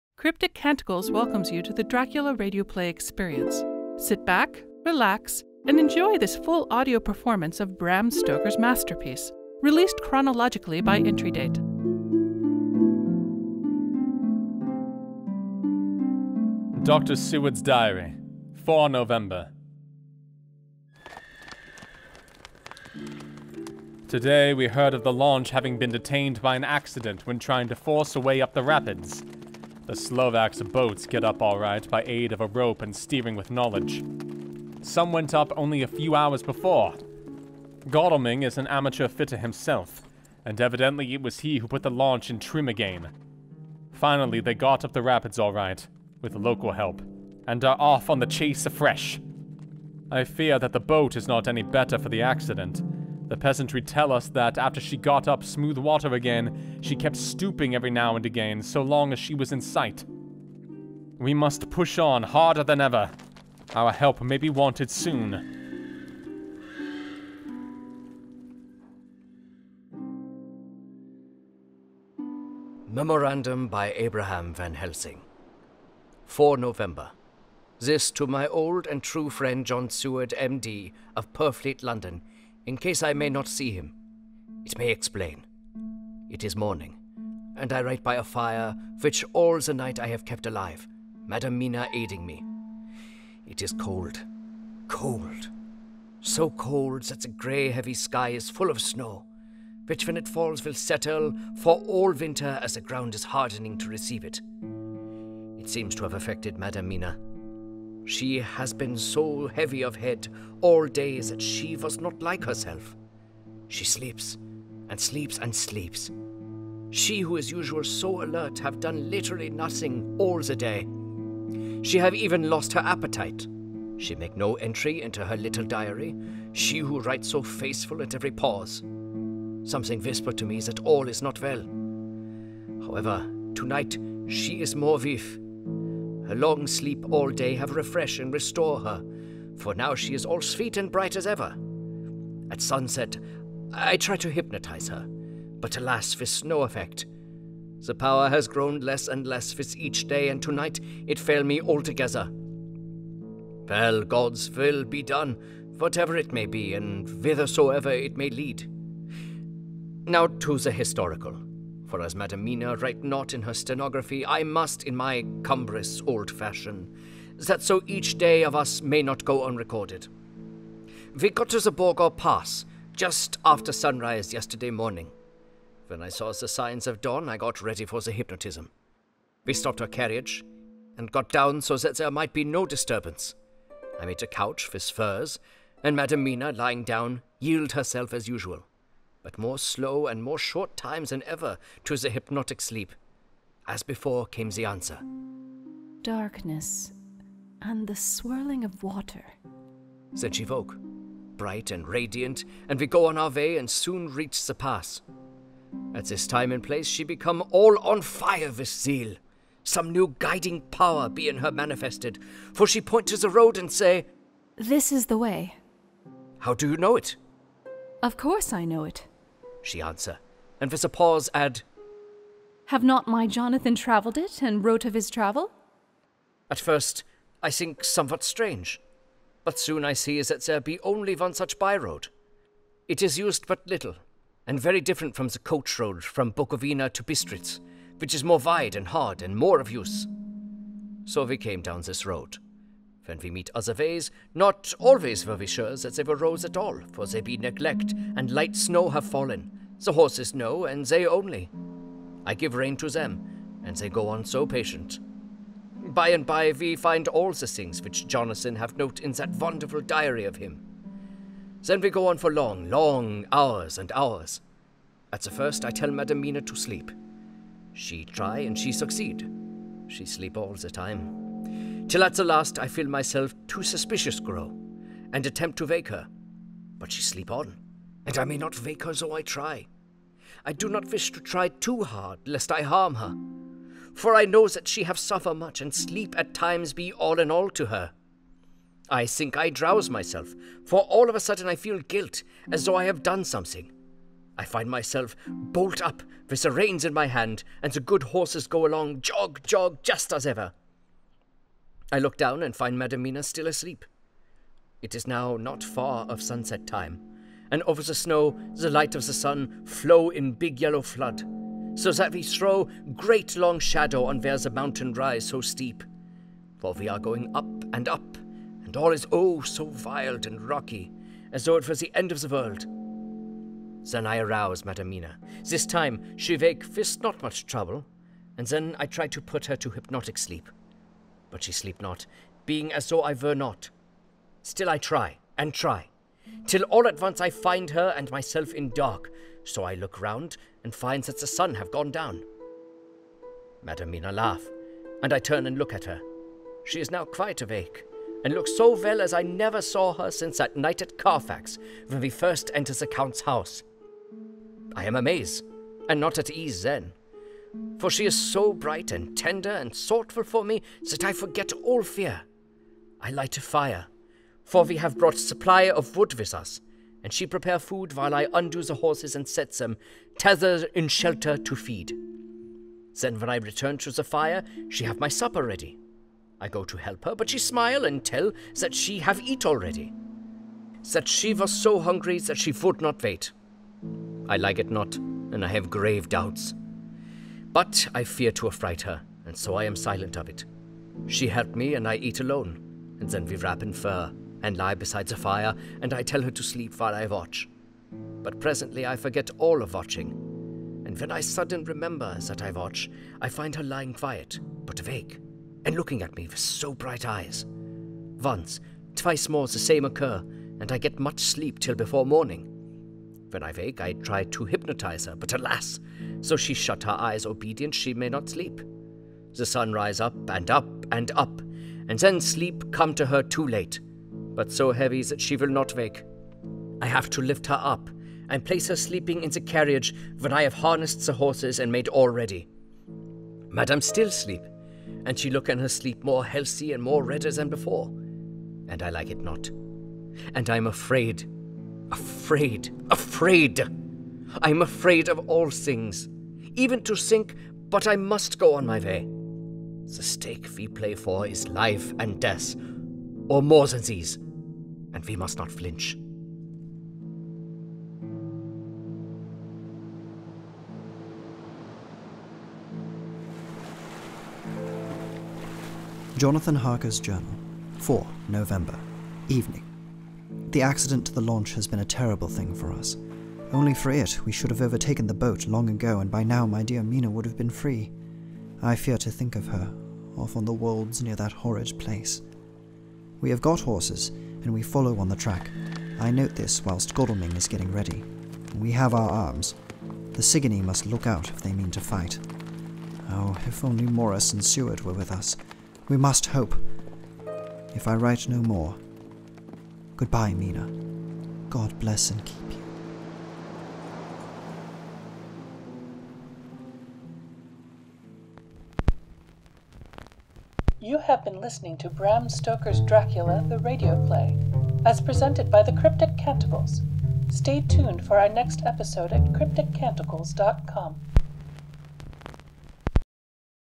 November 4th
Episode Synopsis:

Seward and Quincey hear from the locals that Jonathan and Arthur had encountered trouble navigating some of the rapids in their steamer and need some help righting their vessel.
Van Helsing and Mina reach the Borgo Pass at sunrise. Mina made her same report at sunrise while under hypnosis.  When she awakes, she recognizes the way to the Count’s castle from Jonathan’ description in his diary. Mina sleeps all day, and Van Helsing is unable to hypnotize her at sunset. Mina makes Van Helsing’s diner, but tells him that she has eaten hers already, which makes him nervous. Mina is awake and alert all night, but in the morning, Van Helsing is unable to hypnotize her before sunrise, but she falls into such a deep sleep, he has to lift her into the carriage.  Van Helsing worries about the acceleration of Mina’s affliction.
Jonathan agonizes over the accident that he and Arthur had encountered earlier, surmising that had it not occurred, he believes that they could have overtaken the Count already. He prepares his arms for the possible combat ahead, and writes a preemptive goodbye to Mina should he be unable to write again.

 Cast
Dr Seward: Sean Lenhart
Van Helsing: Cyrus Nemati
Jonathan Harker: Jesse Buddington
Mina Harker: Bonnie Bogovich

Producer, Director, Webmaster: Gwen Schmidt
Audio Engineer, SFX and Music: Bonnie Bogovich
Scripting and Assistant Audio: Sean Lenhart
Second Assistant Audio: Brian Diamond
Story Continuity Supervisor: Liz Rishel
Artwork: Kwamé Babb